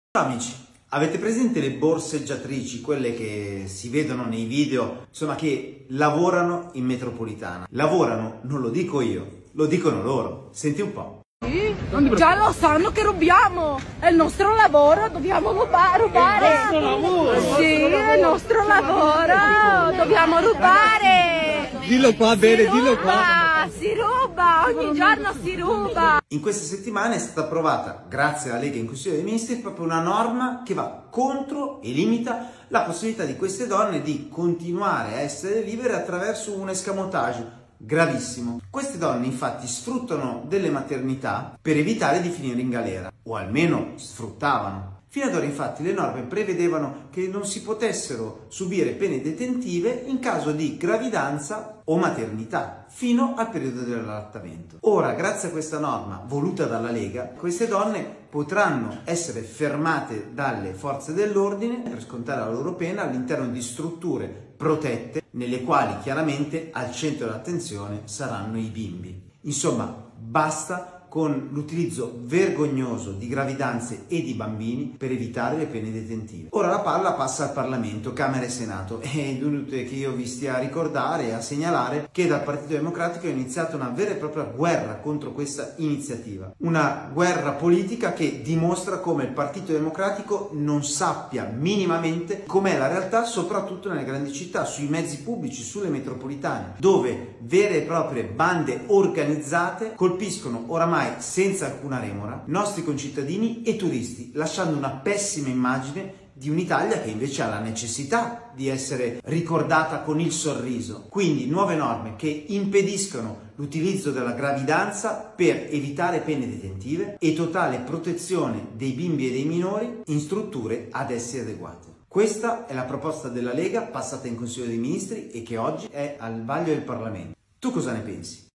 Morelli - Il provvedimento (30.11.23)
- Morelli - Il provvedimento, contenuto nel pacchetto sicurezza, inizia ora il suo iter di approvazione parlamentare. (30.11.23)